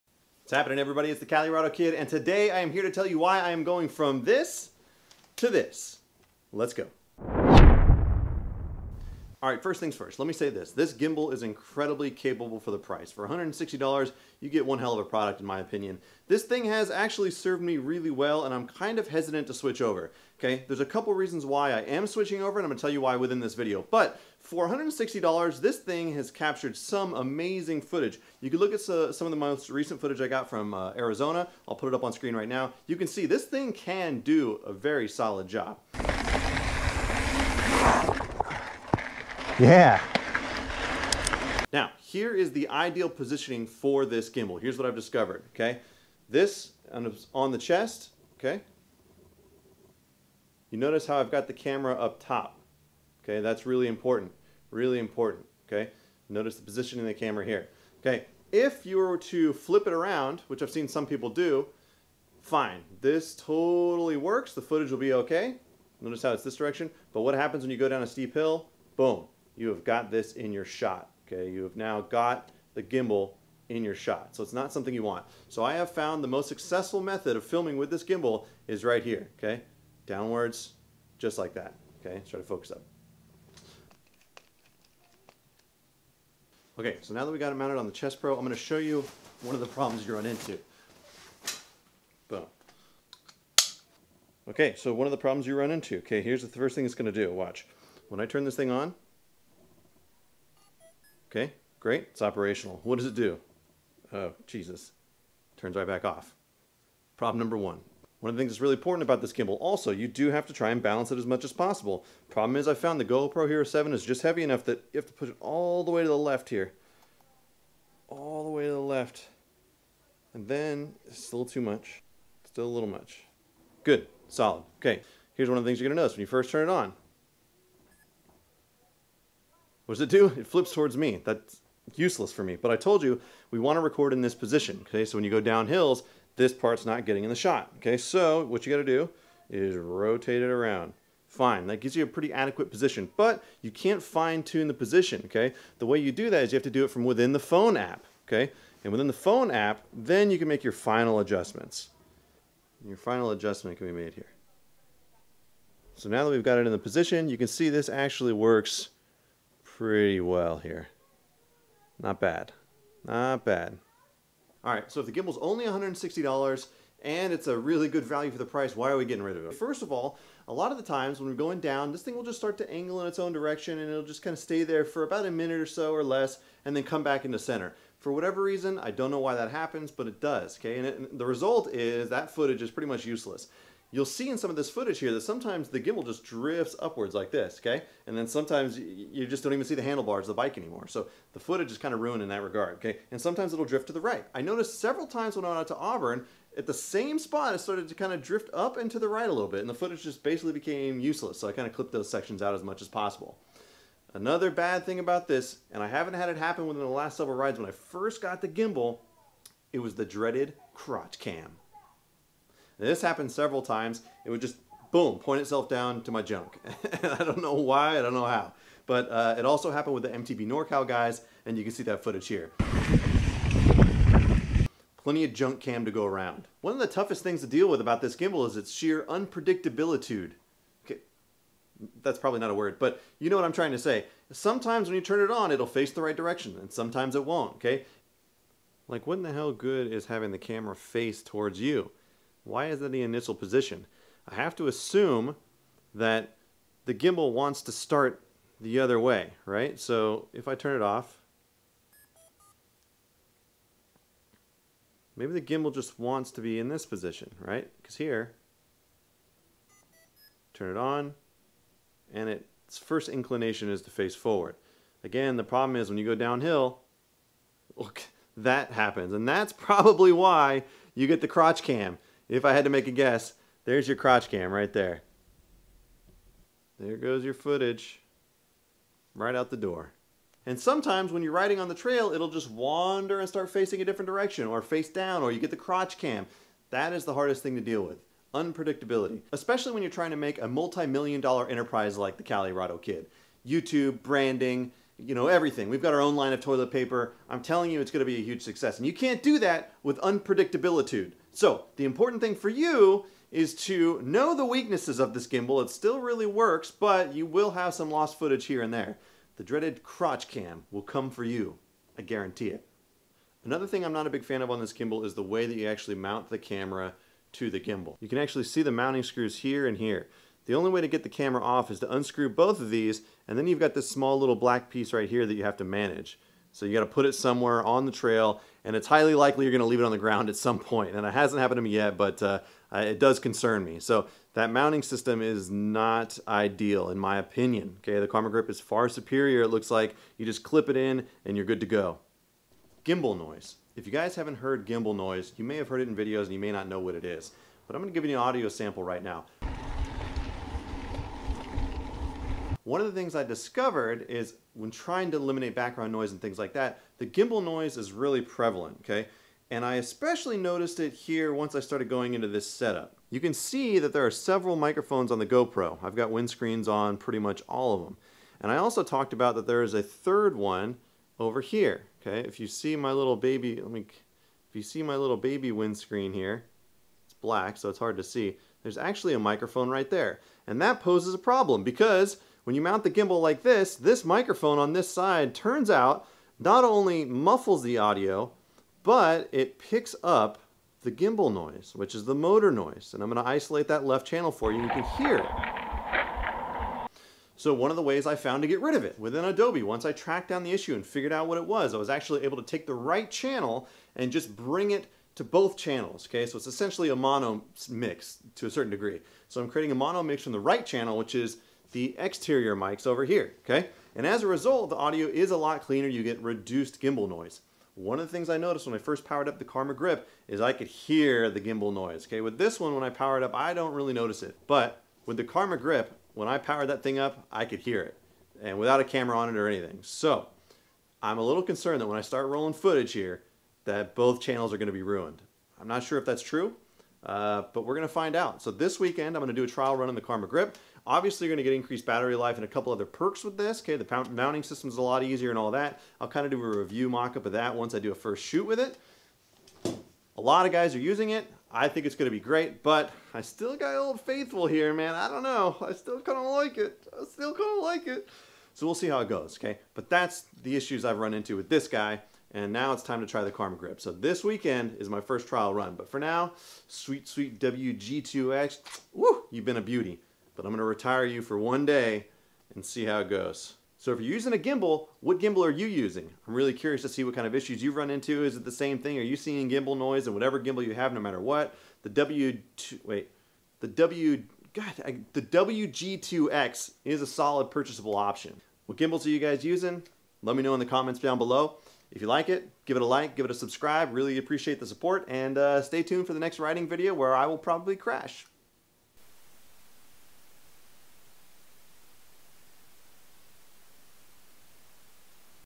I'm kissing the Feiyutech WG2X GOODBYE!?
In my latest video I discuss the reasons why I'm ditching the WG2X for the Karma Grip.  From it's occasional wandering eye to the dreaded "Crotch Shot" I'm hoping the Karma Grip reduces the amount of lost shots I have on a ride.